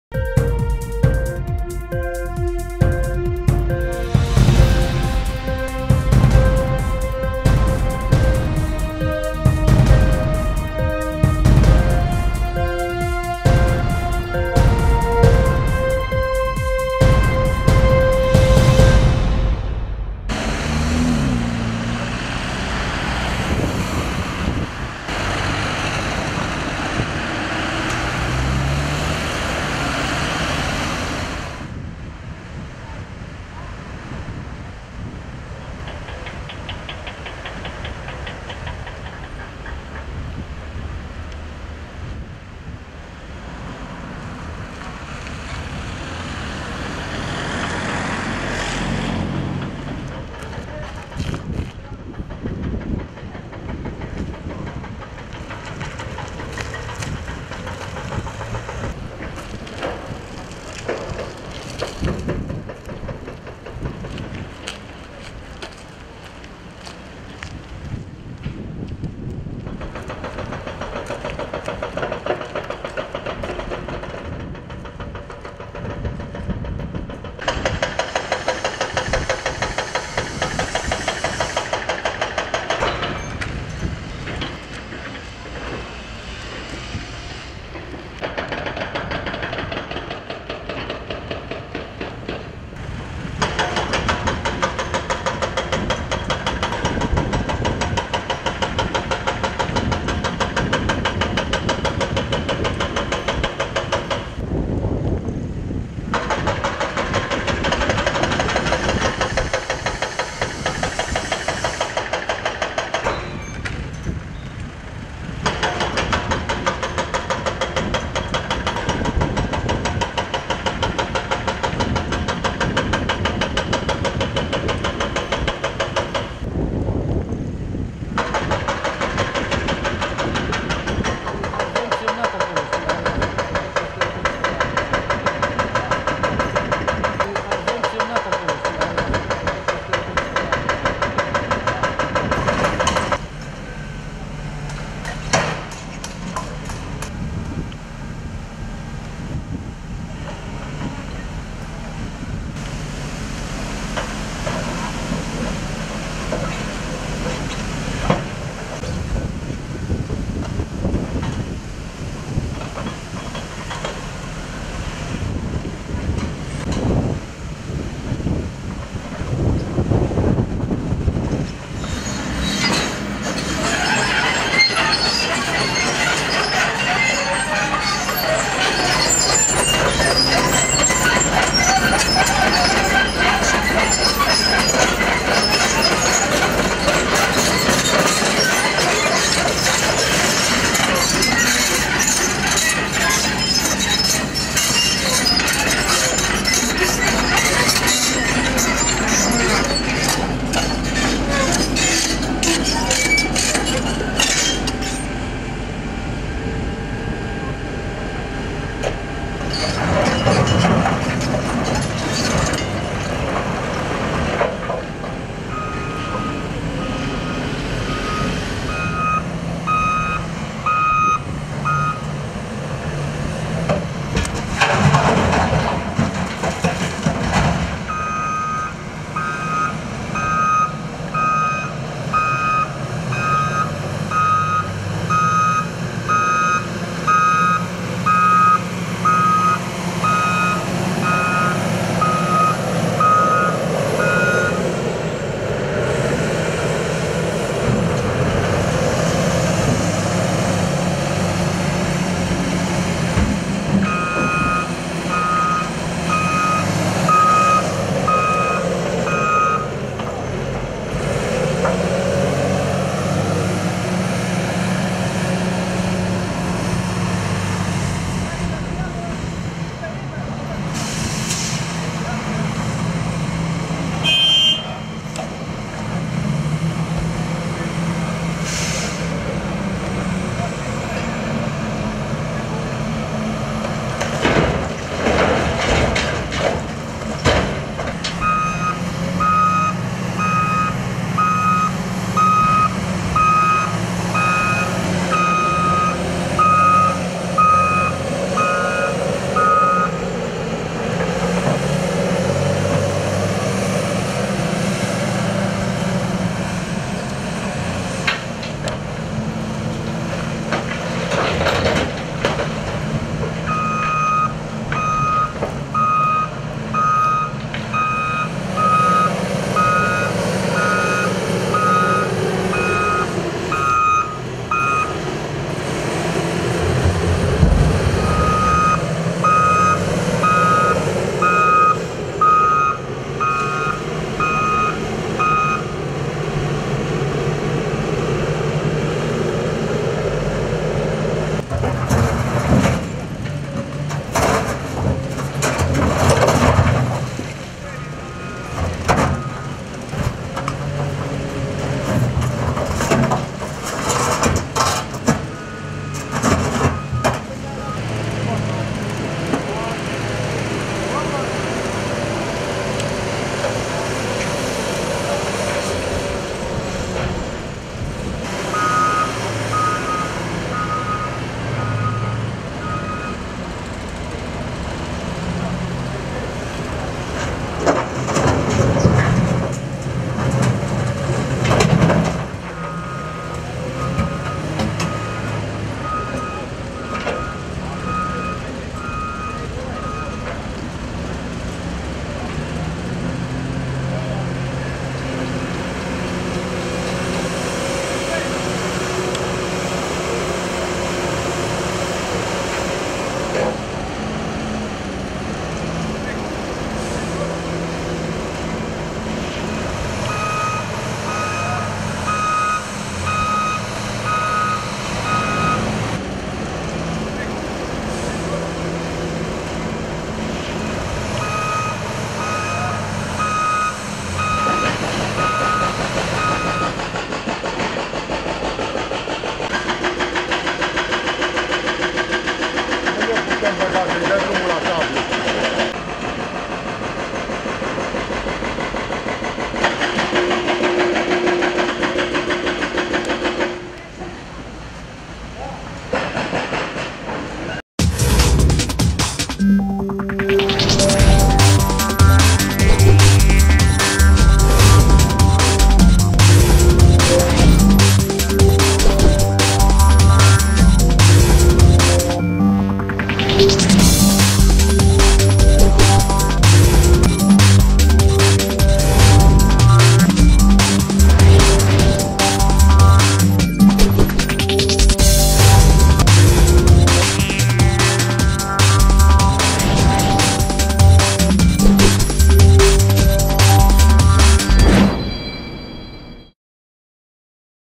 Muncitorii de pe șantierul Popa Șapcă ”pun osul” la treabă!
Lucrările la Pasajul Popa Şapcă au intrat în linie dreaptă! Se lucrează din plin la lărgirea pasajului pe sub calea ferată, dar şi la lărgirea străzii Popa Şapcă de la două la patru benzi iar de la ieşirea din pasaj la şase benzi.